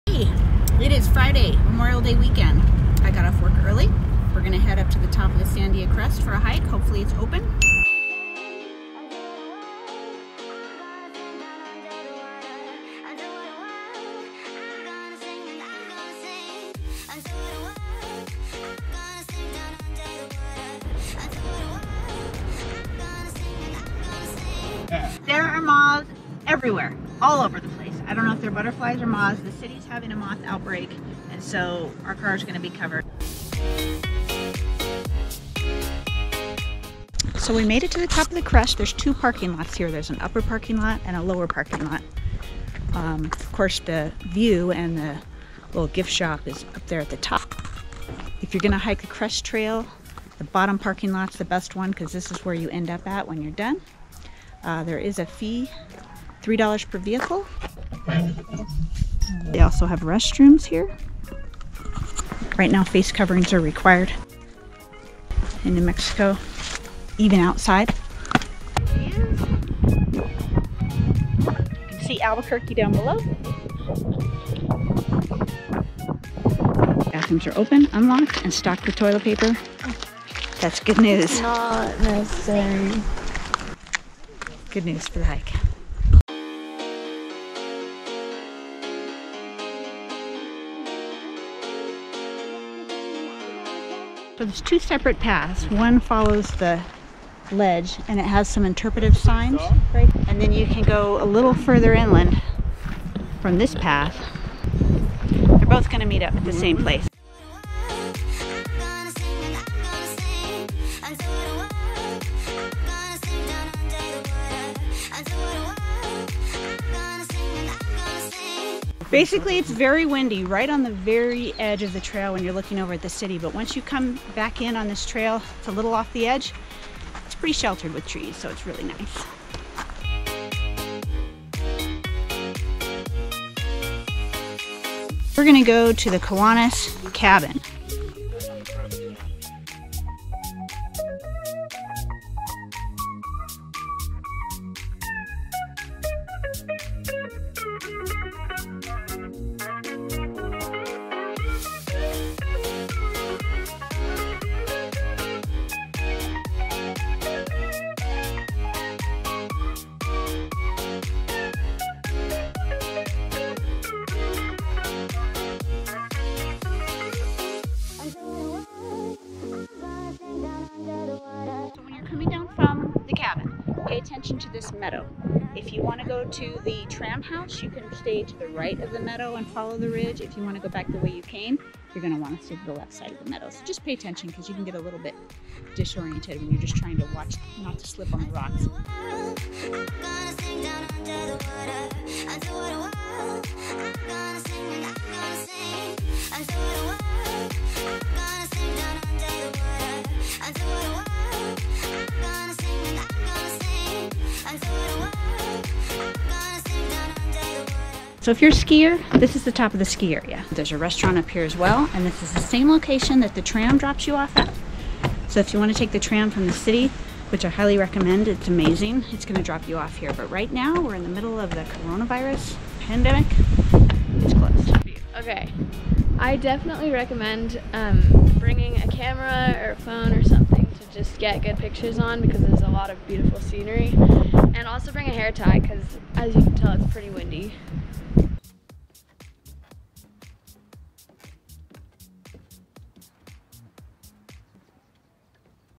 What to do in Albuquerque? Sandia Crest Trail to Kiwanis Cabin & Sandia Tram
What to do in Albuquerque? In this video we drive up to the Sandia Crest and Hike the Crest Trail to Kiwanis Cabin and the Sandia Tram. Join us!

Any questions/comments about this video or anything related to the Albuquerque area?